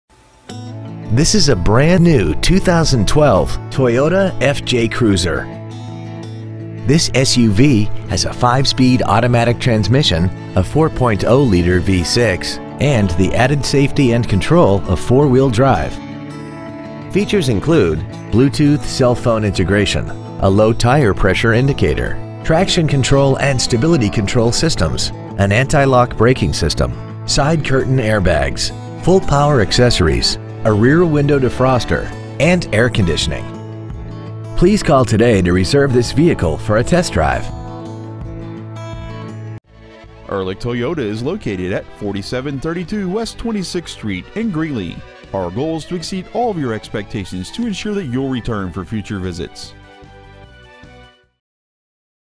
2012 Toyota FJ Cruiser Fort Collins CO
We have been honored to serve the Greeley CO area , we promise that your experience at our dealership will exceed your expectations !

 Year : 2012
 Make : Toyota
 Model : FJ Cruiser
 Engine : 4.0L V6
 Trans . : Automatic 5-Speed
 Exterior : Iceberg

 Interior : Dark Charcoal
 Stock : 123521